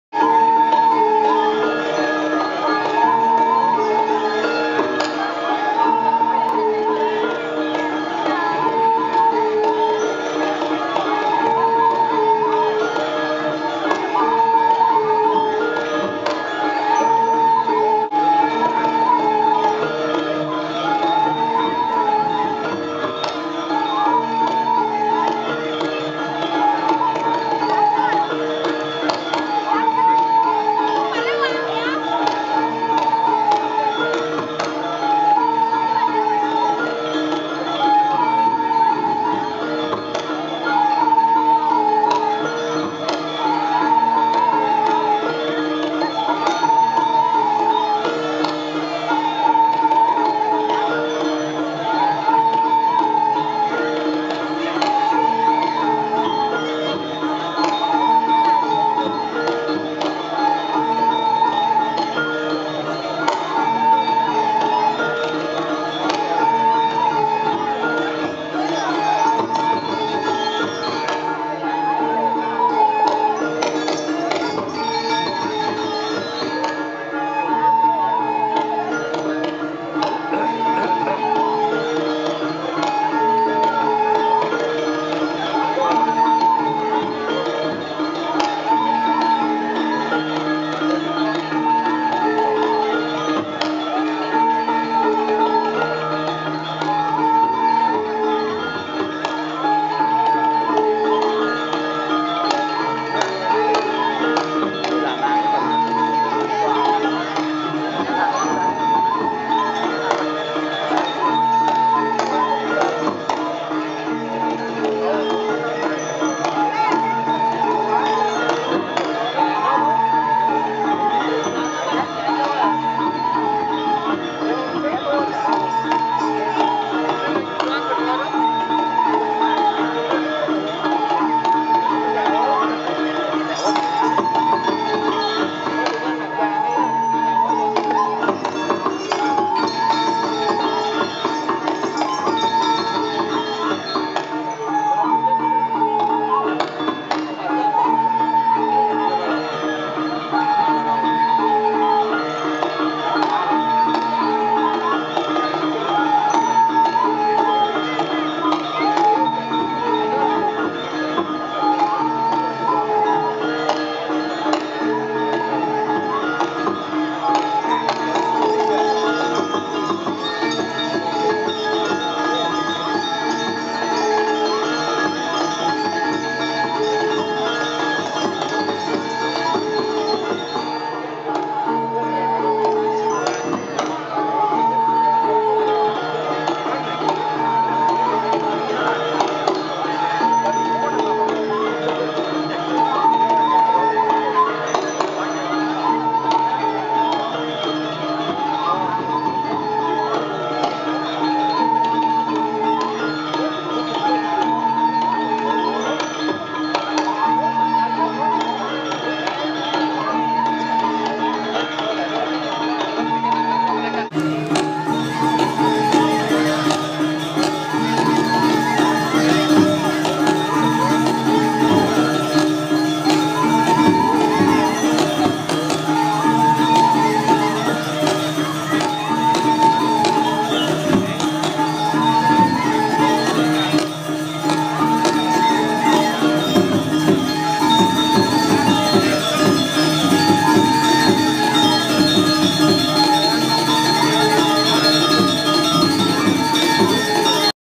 Bali・Barong ・Pura Griya ＠ Ubud
「アパ？情報センター」下働き伊藤博史がお届けする動画集。

Odalan（オダラン＝寺院祭礼）
奉納芸能「チャロナラン舞踊劇」
※バロンが舞台に登場するところを、幕の裏から撮ってみました。
幕を一つ隔てたただけで、場の緊張感が違う。
■場所：Pura Griya Snyar @ Tegallalang

（撮影：2017/9/19）